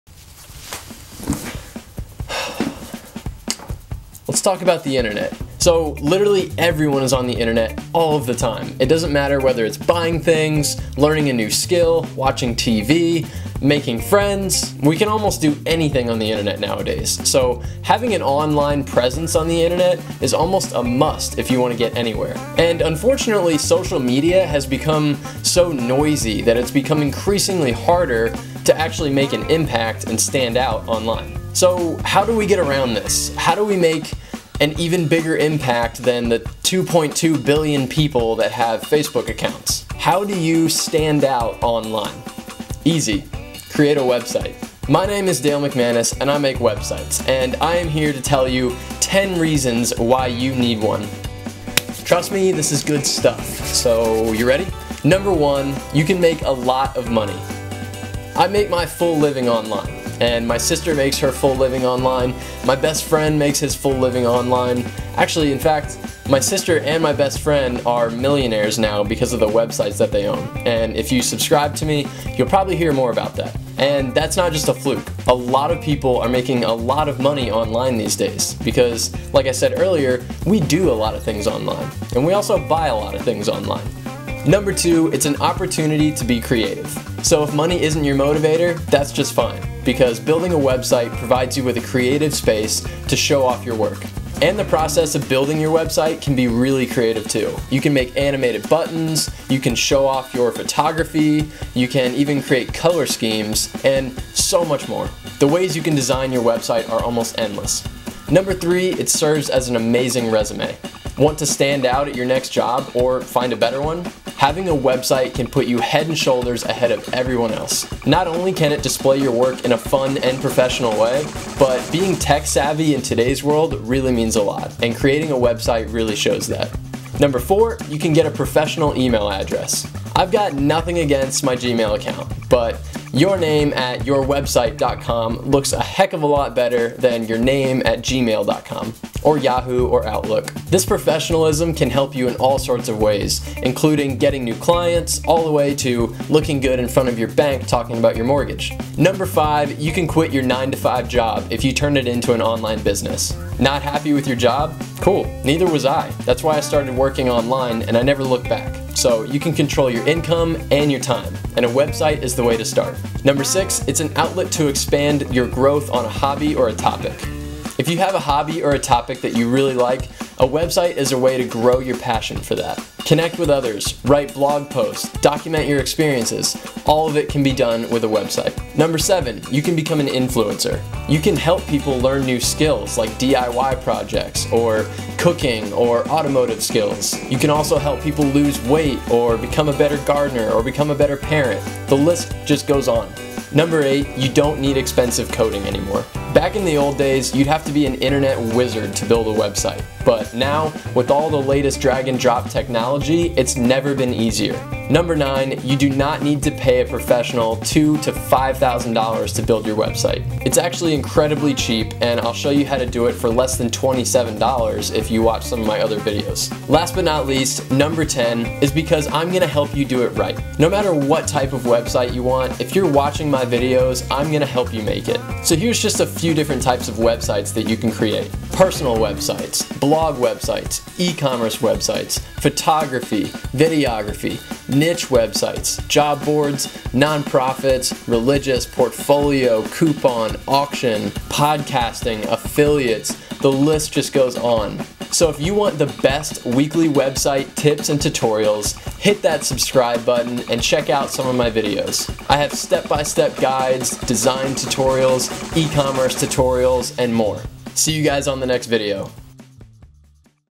10 Reasons You NEED a Website
Hey guys, just wanted to throw a quick video up about 10 reasons why you need a website, in case you're unaware of the awesome advantages! I know I was a few years ago, and now my life has completely (and literally) changed. Wanna control your income or work from home? How about land that next job interview? Maybe become an influencer?